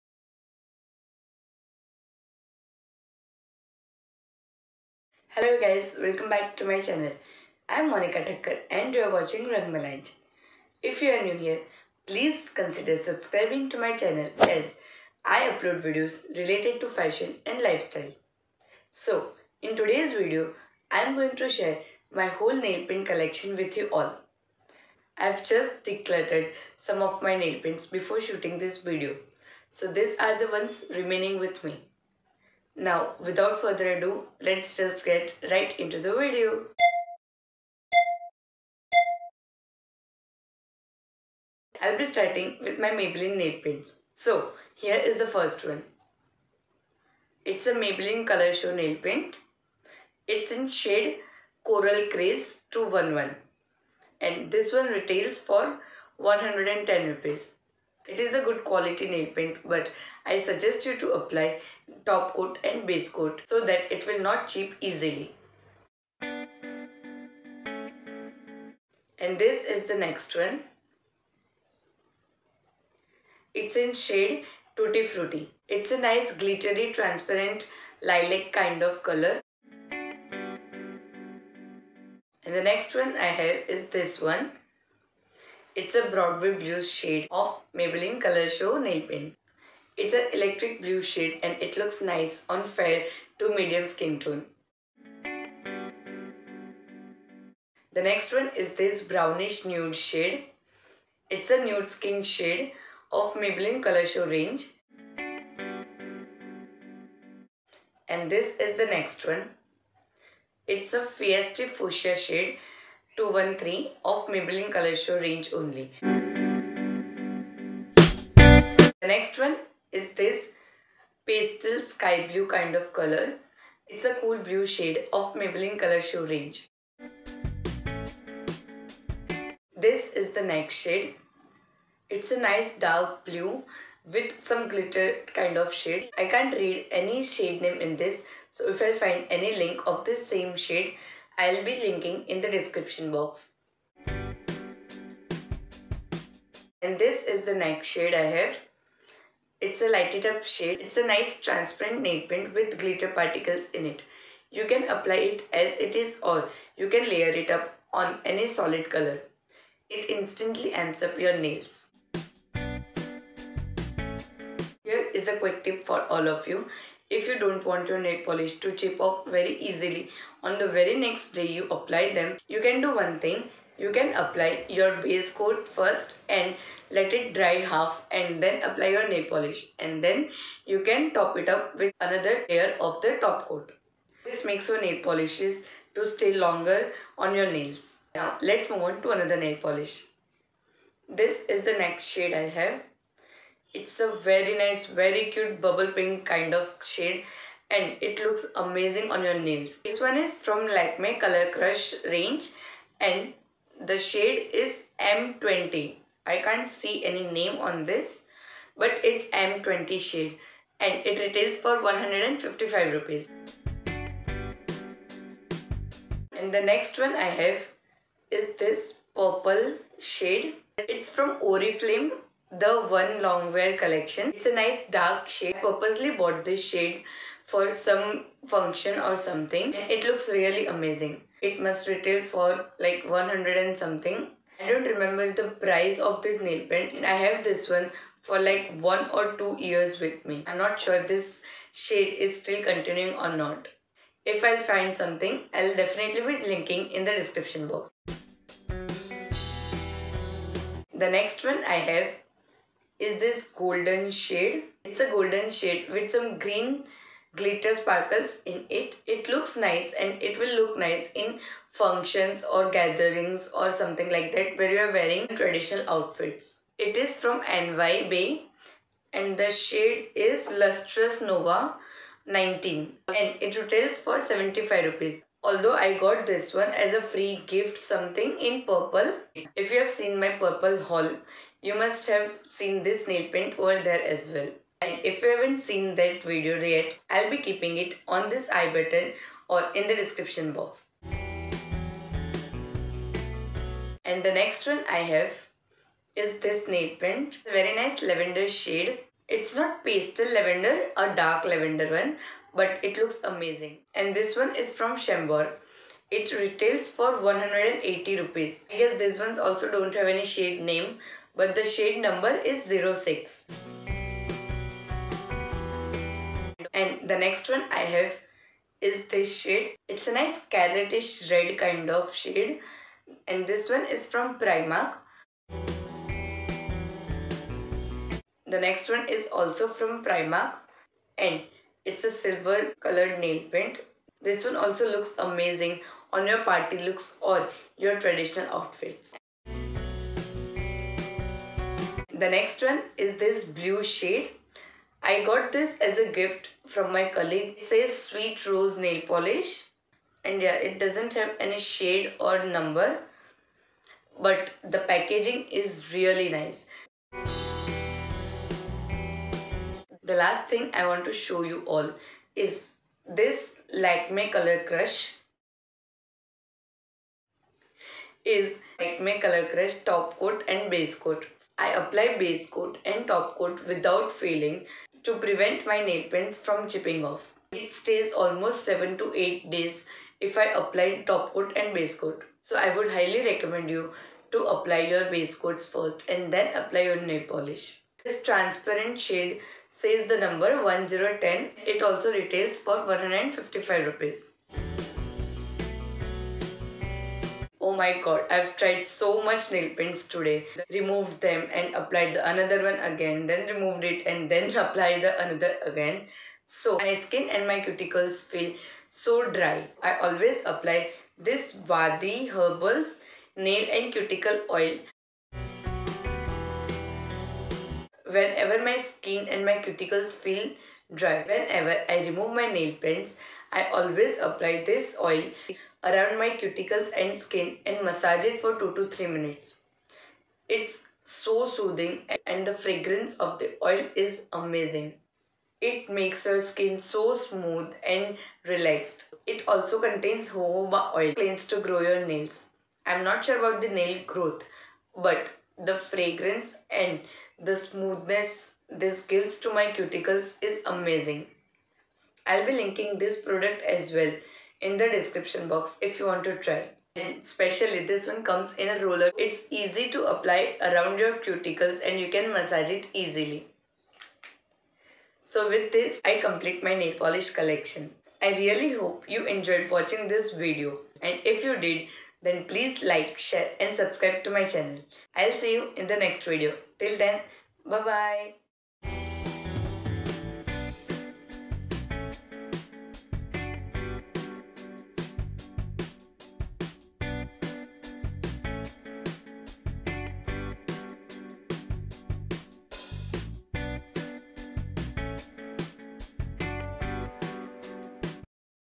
MY NAIL PAINT COLLECTION 2020 | MAYBELLINE | LAKME | CHAMBOR | NY BAE | ORIFLAME | PRIMARK + TIPS
Hello everyone in today's video I am sharing my current Nail Paint collection. The collection is built over 3-4 years. I hope you all like the video & enjoyed watching it.  KEEP WATCHING !!!

Nail Paints shown in video (available ones only)

Maybelline Broadway Blues

Maybelline Coral Craze

Maybelline Nude Skin

Maybelline Feisty Fuchsia

Maybelline Cool Blue

Maybelline Ladies Night

Oraflame (another shade from same range)

Lakme Top Coat